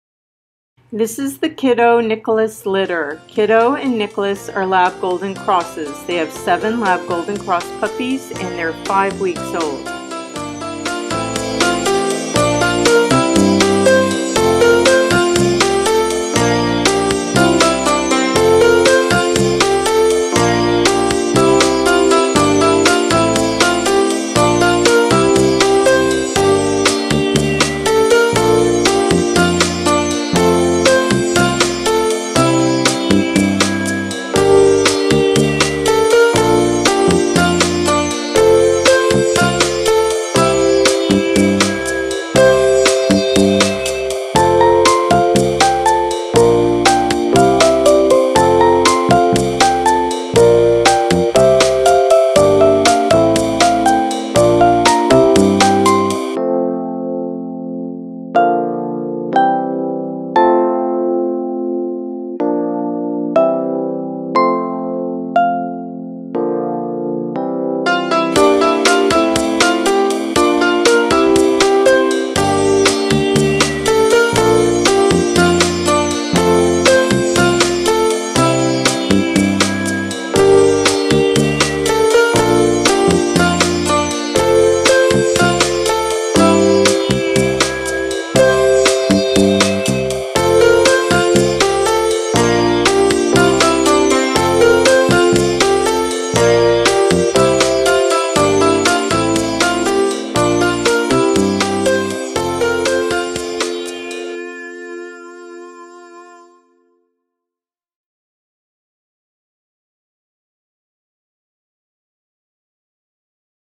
04-01-24 Kiddo
Kiddo x Nicholas Litter 7 LGX puppies Five weeks old DOB: 03-01-24